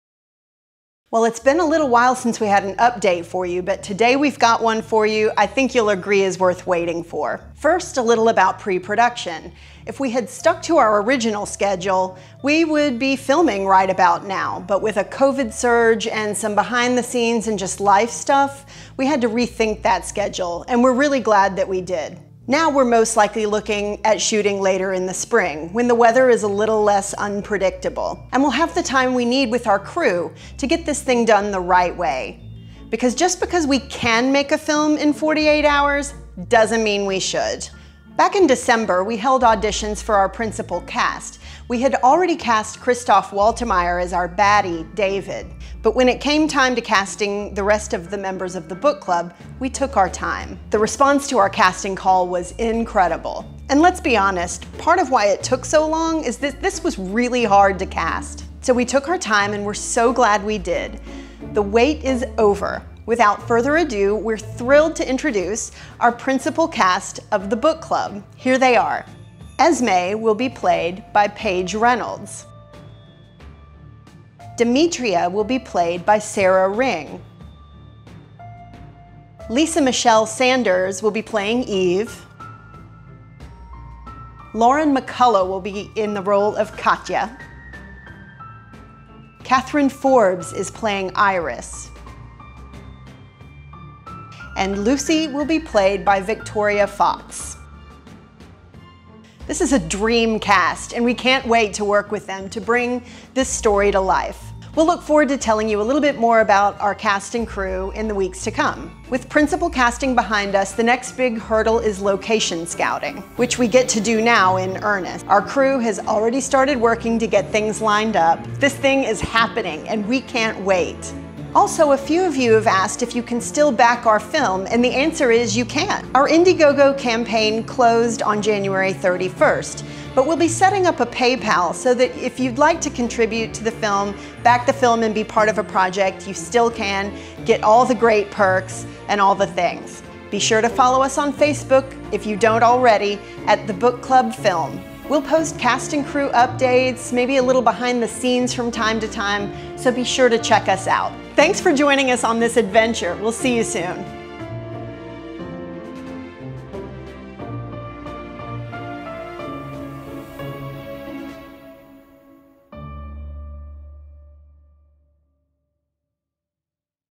The Book Club Short Film | Week 4 | Update | Meet The Cast!
In this latest update, we FINALLY get to spill the beans. Meet the INCREDIBLE cast of The Book Club!

Camera - SONY A6400
Sound - Rode Wireless GO
Purple Panda Lavalier Lapel Microphone Kit
Light Kit - GVM 672S